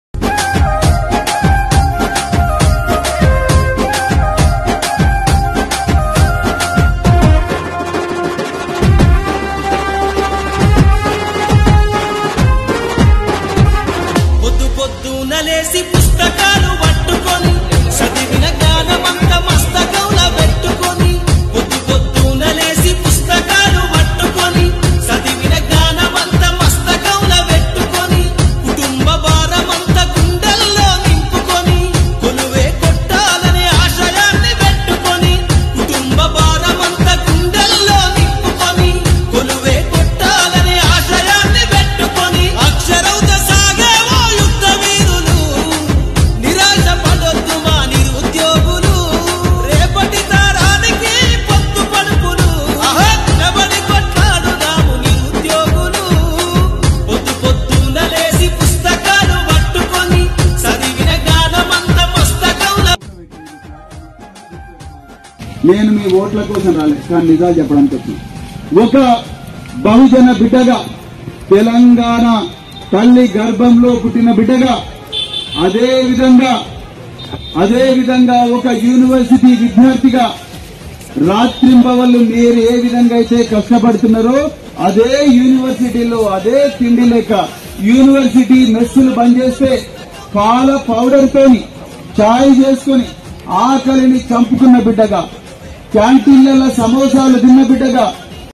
హన్మకొండ జిల్లా కాకతీయ యూనివర్సిటీలో నిరుద్యోగుల గోస - RSP భరోసా యూనివర్సిటీ విద్యార్థుల
హన్మకొండ జిల్లా కాకతీయ యూనివర్సిటీలో నిరుద్యోగుల గోస - RSP భరోసాలో యూనివర్సిటీ విద్యార్థుల తరుపున నికబడి కొట్లాడుతున్న ఏకైక నాయకుడు